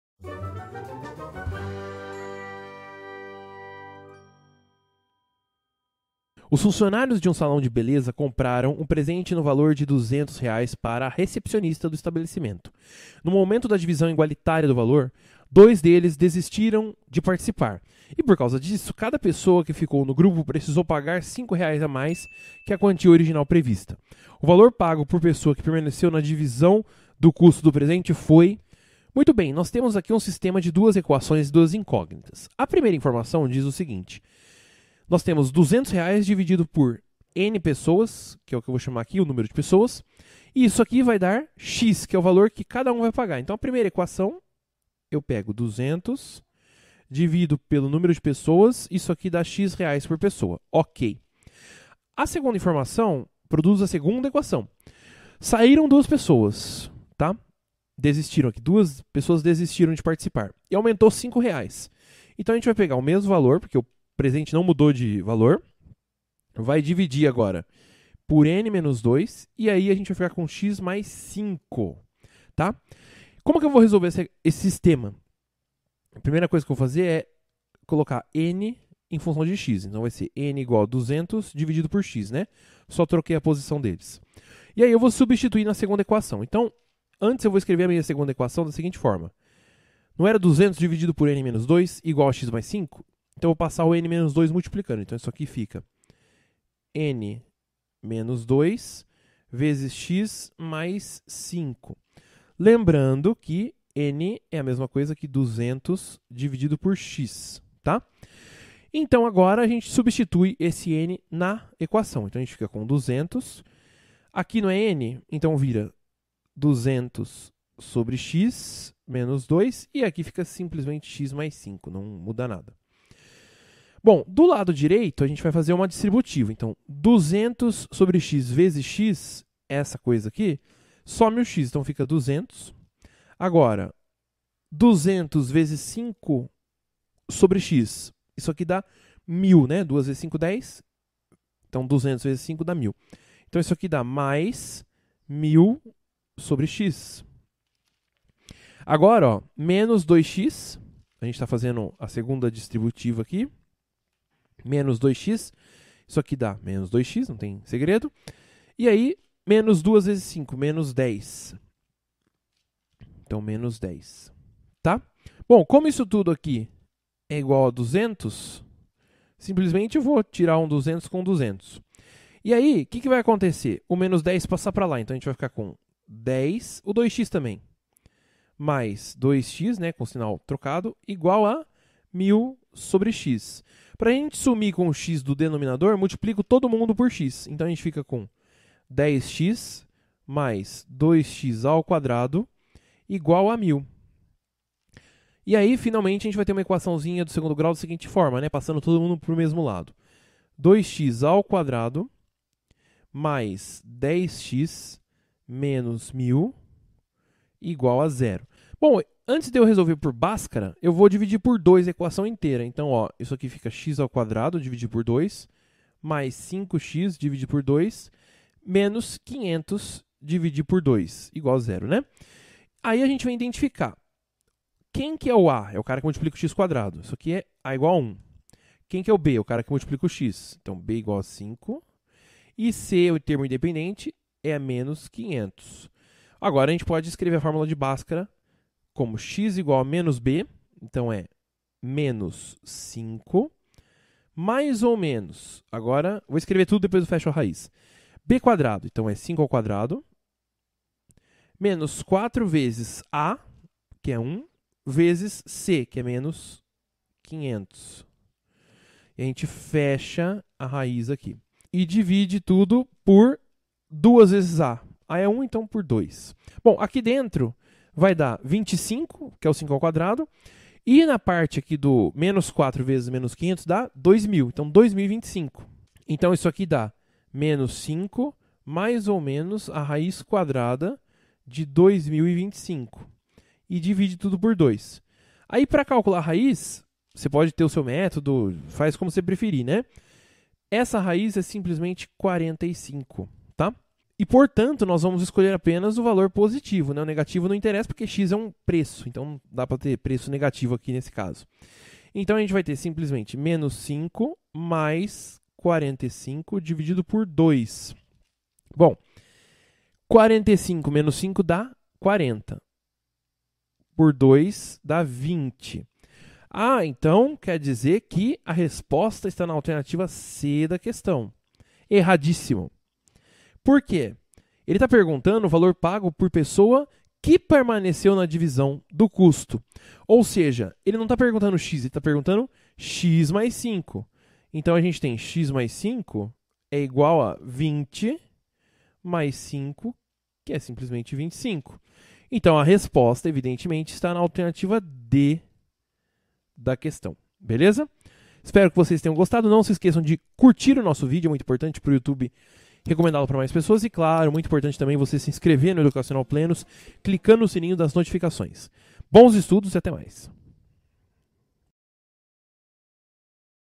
FUVEST 2022 - Os funcionários de um salão
Os funcionários de um salão de beleza compraram um presente no valor de R$ 200,00 para a recepcionista do estabelecimento. No momento da divisão igualitária do valor, dois deles desistiram de participar e, por causa disso, cada pessoa que ficou no grupo precisou pagar R$ 5,00 a mais que a quantia originalmente prevista. O valor pago por pessoa que permaneceu na divisão do custo do presente foi: